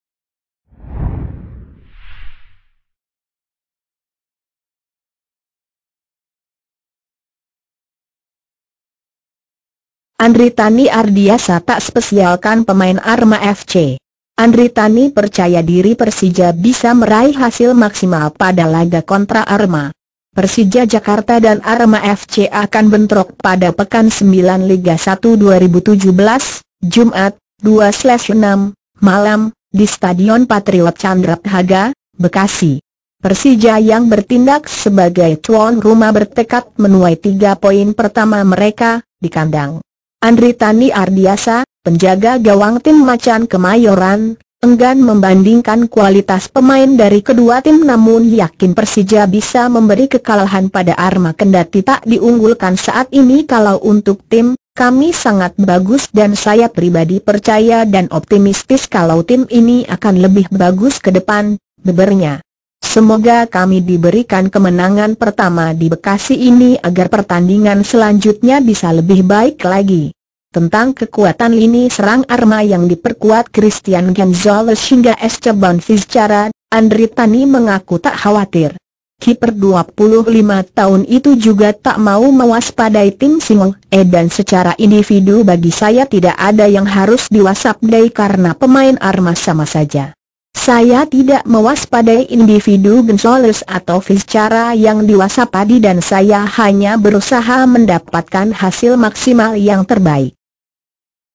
Andritany Ardhiyasa Tak Spesialkan Pemain Arema FC
Andritany percaya diri Persija bisa meraih hasil maksimal pada laga kontra Arema.

Persija Jakarta dan Arema FC akan bentrok pada pekan sembilan Liga 1 2017, Jumat (2/6) malam, di Stadion Patriot Candrabhaga, Bekasi. Persija yang bertindak sebagai tuan rumah bertekad menuai tiga poin pertama mereka, di kandang.

Andritany Ardhiyasa, penjaga gawang tim Macan Kemayoran, enggan membandingkan kualitas pemain dari kedua tim namun yakin Persija bisa memberi kekalahan pada Arema kendati tak diunggulkan saat ini
"Kalau untuk tim, kami sangat bagus dan saya pribadi percaya dan optimistis kalau tim ini akan lebih bagus ke depan," bebernya. "Semoga kami diberikan kemenangan pertama di Bekasi ini agar pertandingan selanjutnya bisa lebih baik lagi."

Tentang kekuatan lini serang Arema yang diperkuat Cristian Gonzales hingga Esteban Vizcarra, Andritany mengaku tak khawatir. Kiper 25 tahun itu juga tak mau mewaspadai tim Singo Edan secara individu
"Bagi saya tidak ada yang harus diwasapdai karena pemain Arema sama saja. Saya tidak mewaspadai individu Gonzales atau Vizcarra yang diwasapadi dan saya hanya berusaha mendapatkan hasil maksimal yang terbaik."